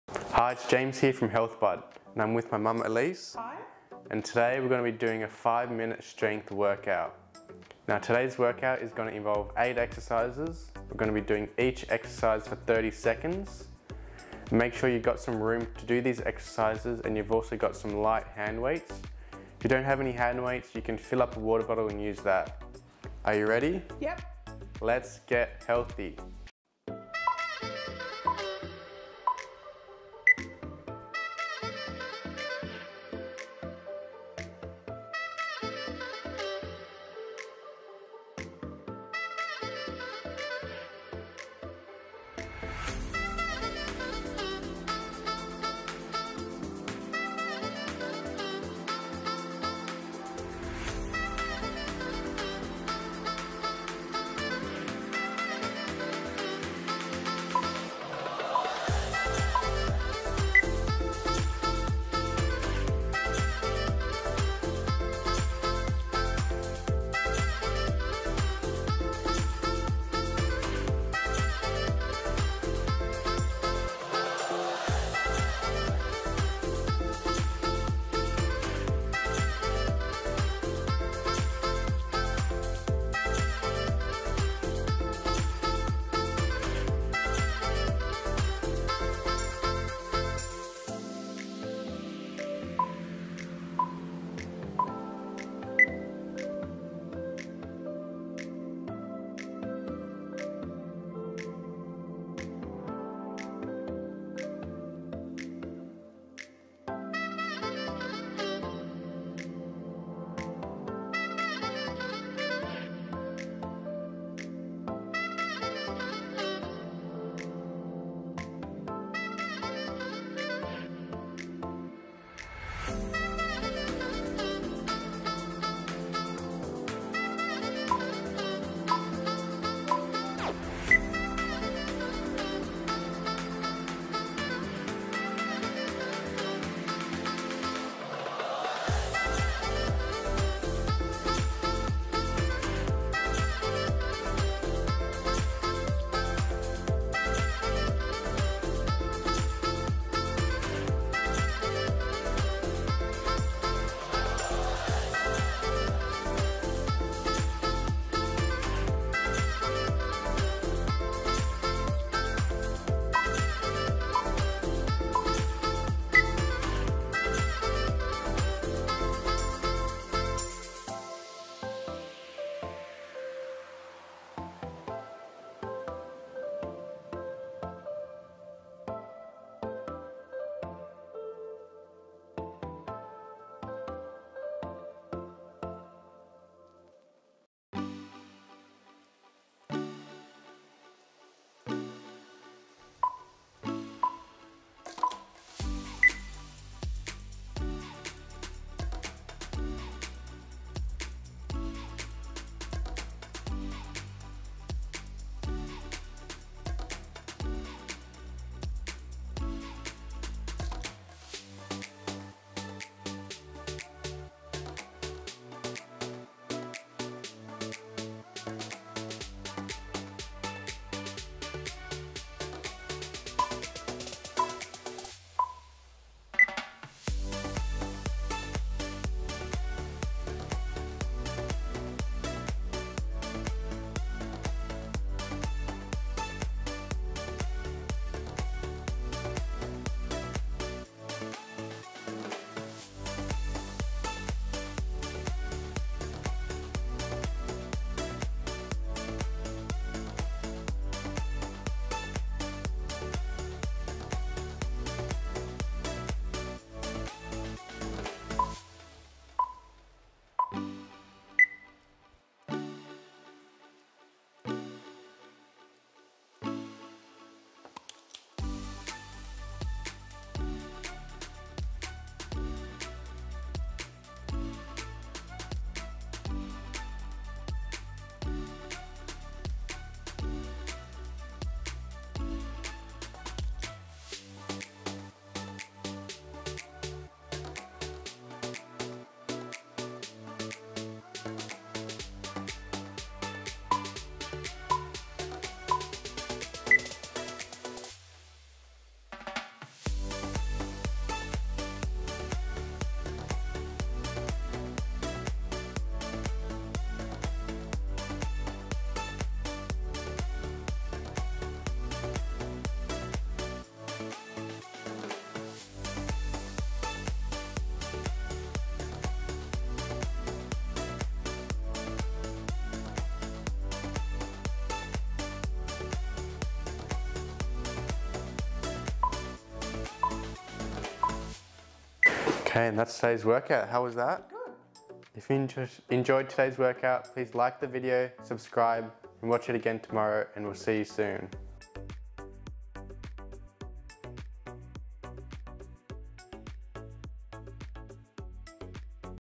TRY THIS 5 Minute Strength Workout For Beginners at Home
Try this 5-minute strength workout at home. Health Budd welcomes all levels of fitness, start watching this video and build the foundations of your fitness journey with this 5-minute strength workout! Enjoy

Then say hello in the COMMENTS we’d love to hear from you!